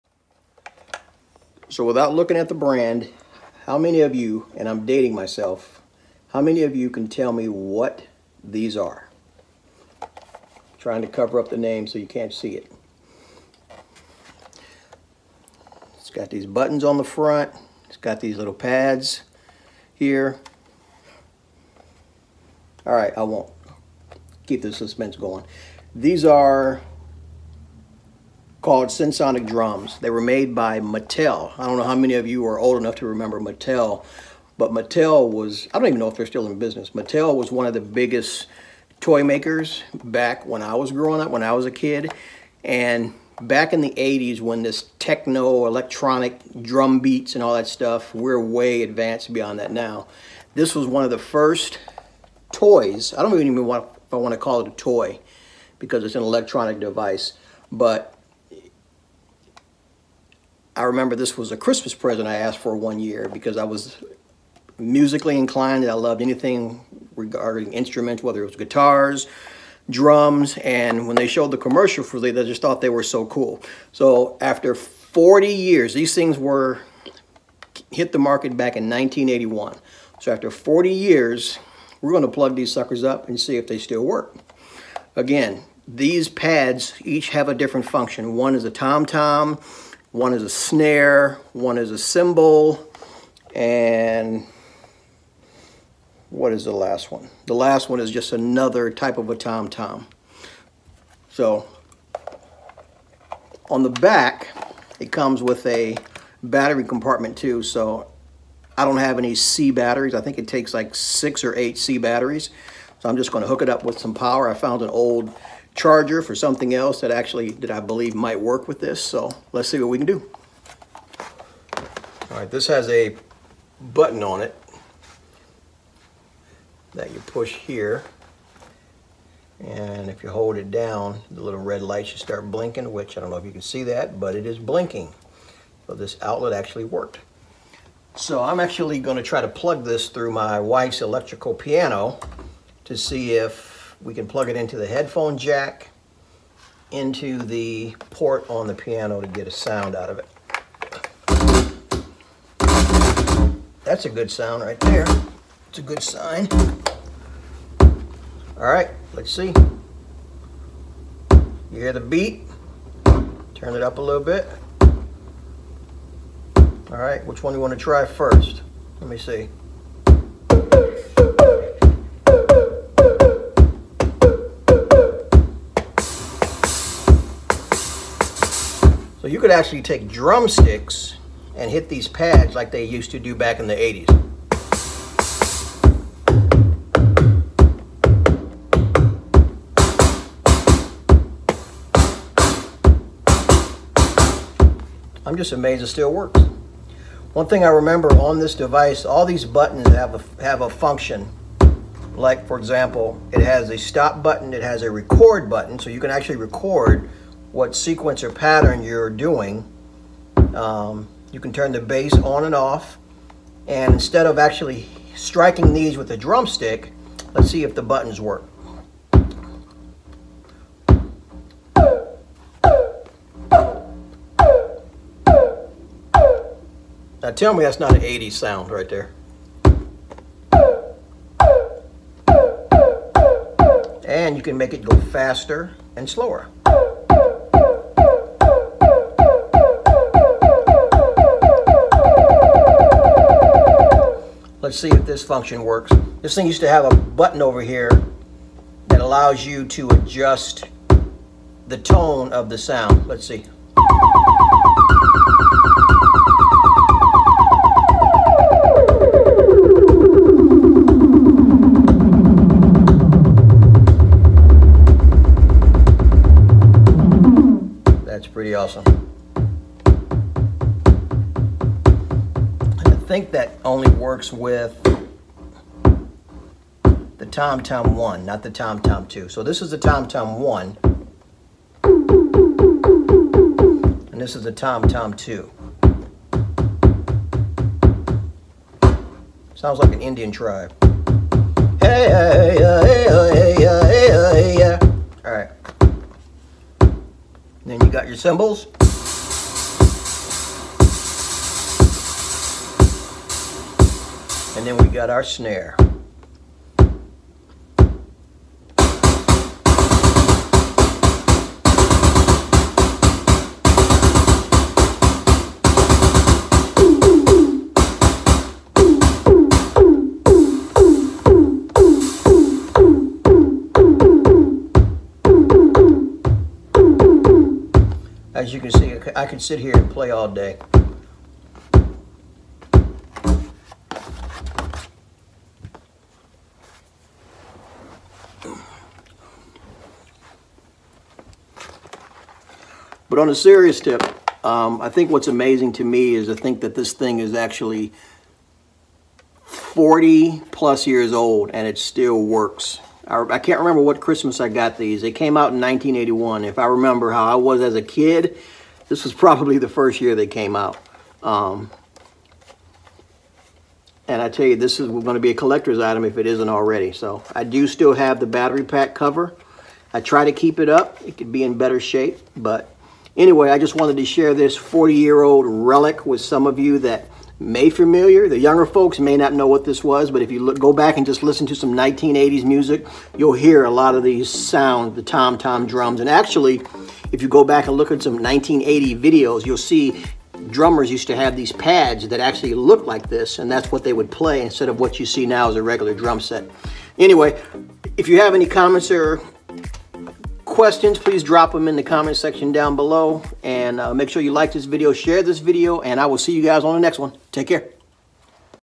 Synsonic Drums A 1981 Collectors Item
In this video I bring out my 1981 collectors item that I have had since Christmas age 15. They are Synsonic drums made by Mattel Electronics and believe it or not they still work. I found an adapter that actually works and was able to plug them through my electronic piano. Hope you enjoy this because they are not around anymore. Hope you enjoy the Synsonic Drums A 1981 Collectors Item.

👉🏽Tabletop 7 Pad Digital Drum Kit